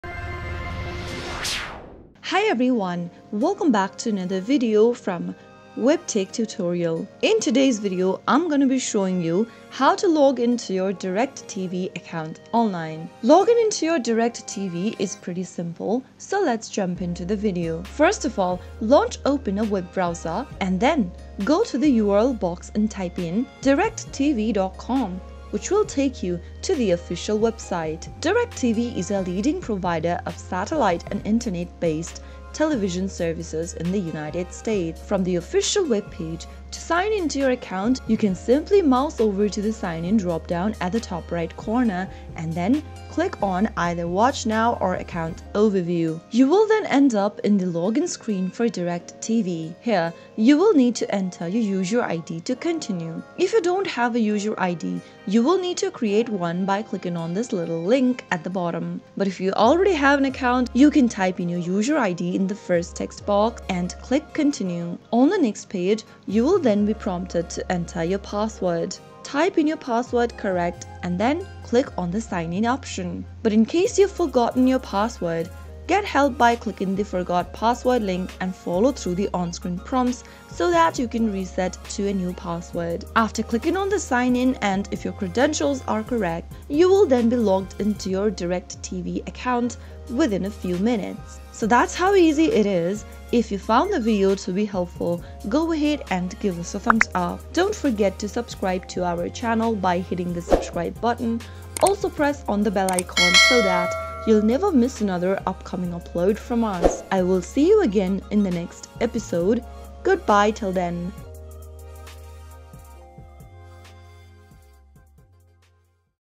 How to Login Sign In DIRECTV Account? Directv.com Login 2024
Ensure uninterrupted access to your favorite shows and channels with our easy-to-follow tutorial on how to log in to DIRECTV. In this video, we'll guide you through the steps, making sure you enjoy a smooth and hassle-free login experience.